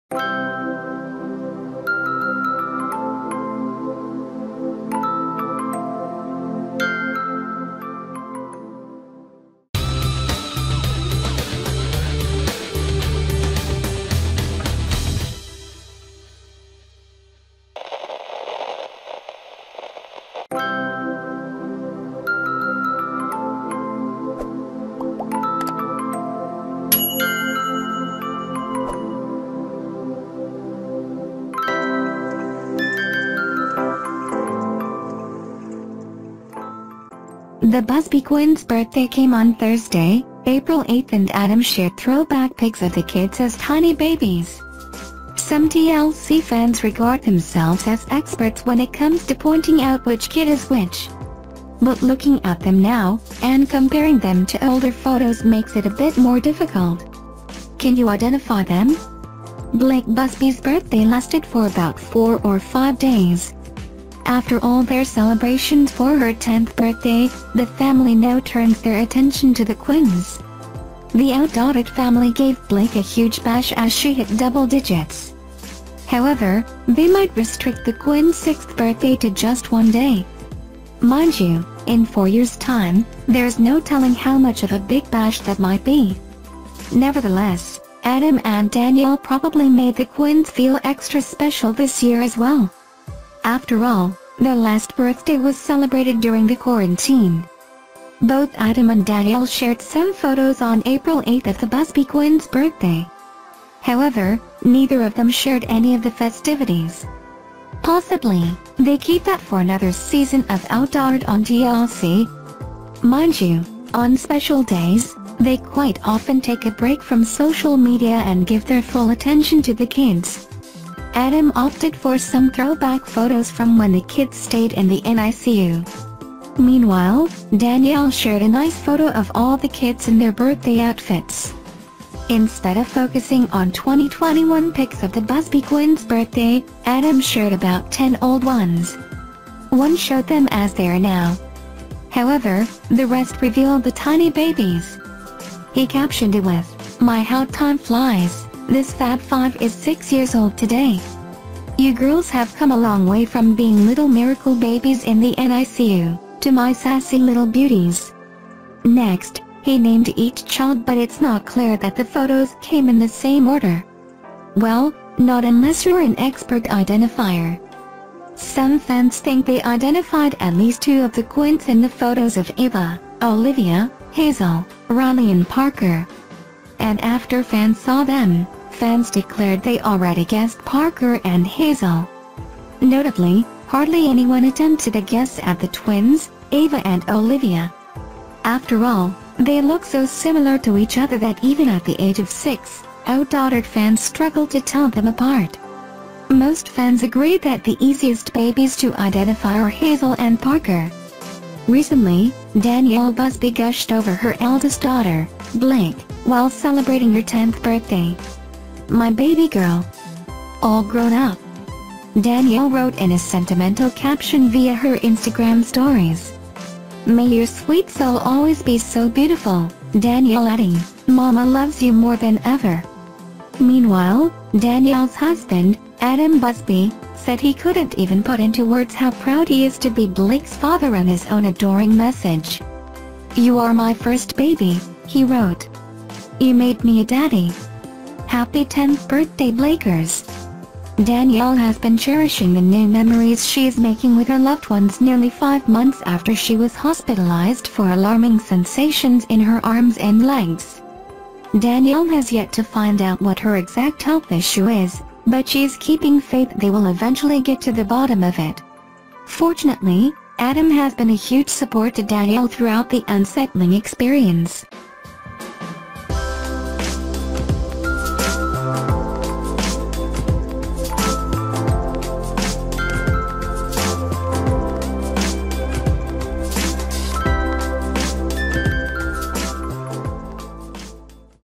HAPPY BIRTHDAY!!! 'OUTDAUGHTERED': THE BUSBY QUINTS TURNING 6 NOW!!!!
The OutDaughtered quints birthday came on Thursday,  Adam shared throwback pics of the kids as tiny babies. Some TLC fans regard themselves as experts when it comes to pointing out which kid is which. But looking at them now, and comparing them to older photos makes it a bit more difficult. Both Adam and Danielle shared some photos on April 8 of the Busby quints birthday. However, neither of them shared any of the festivities.